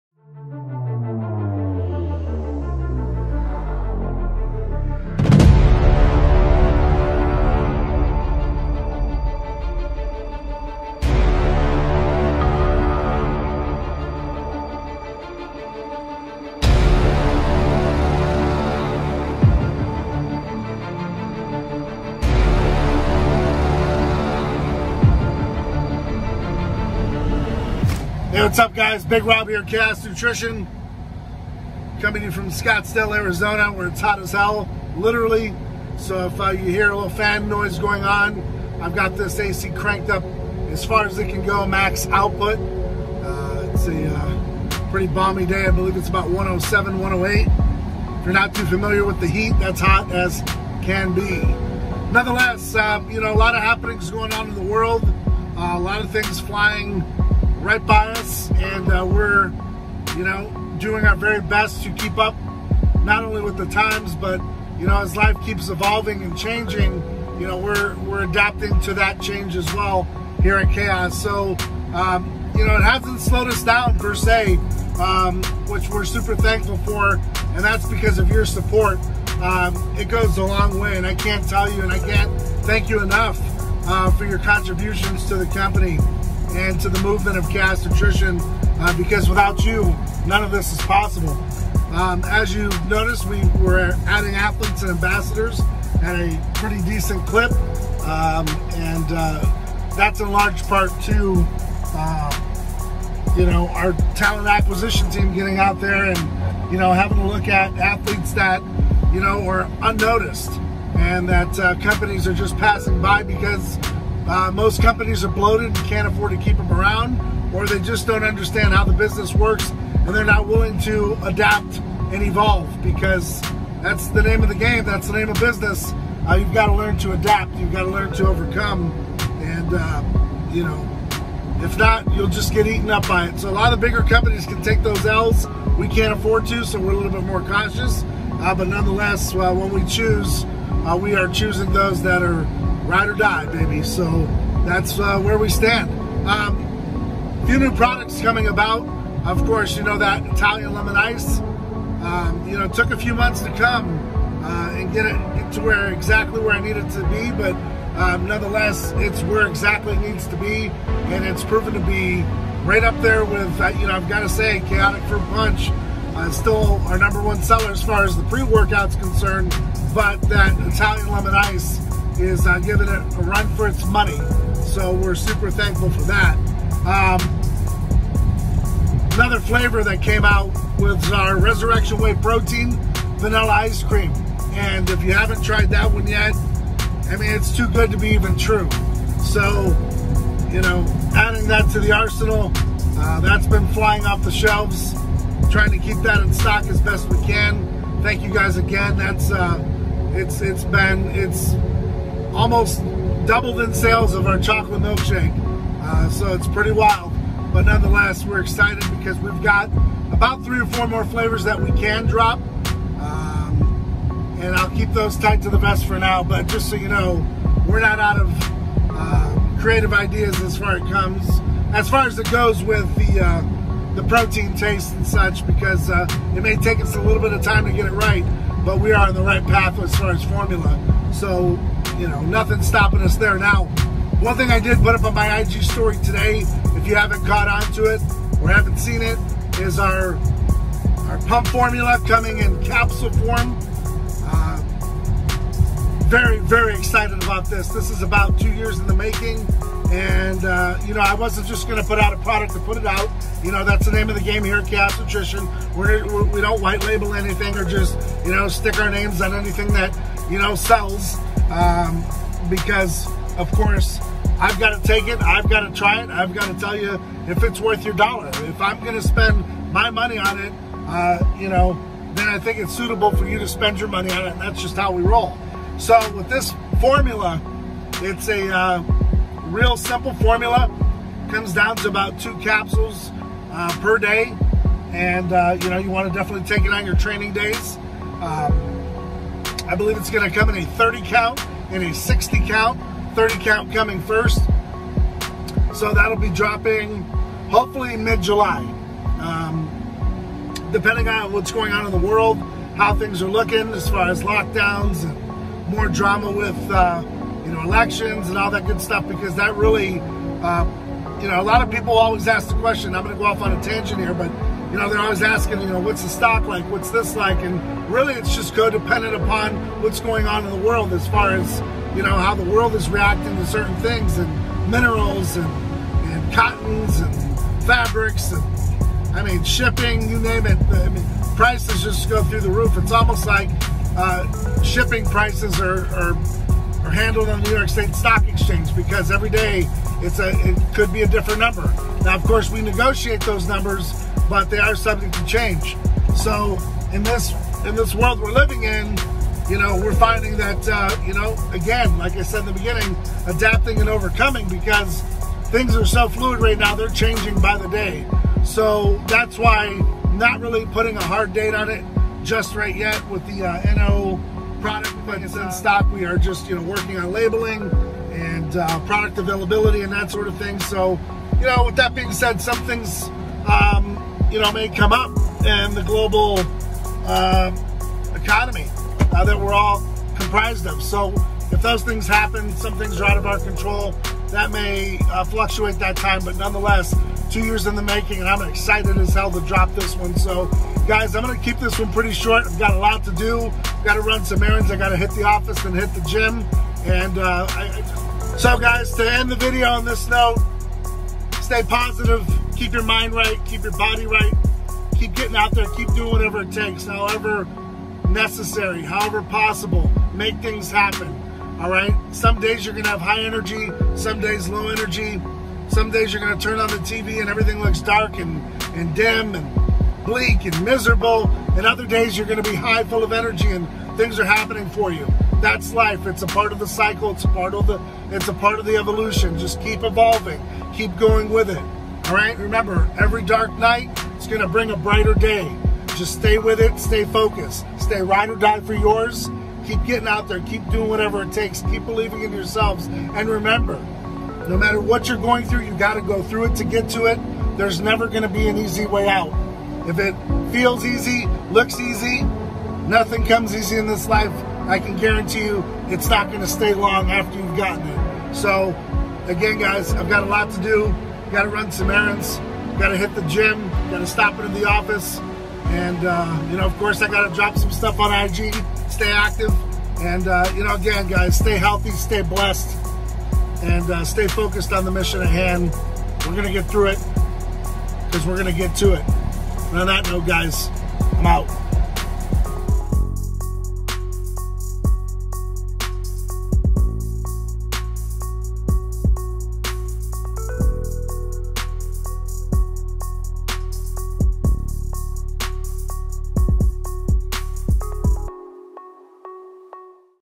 🛑BIG ROB TALKS KEYS TO SUCCESS | NEW ATHLETES | NEW PRODUCTS | EVERYDAY MOTIVATION
US Marine Corps Veteran Big Rob TALKS NEW ATHLETES | NEW PRODUCTS | EVERYDAY MOTIVATION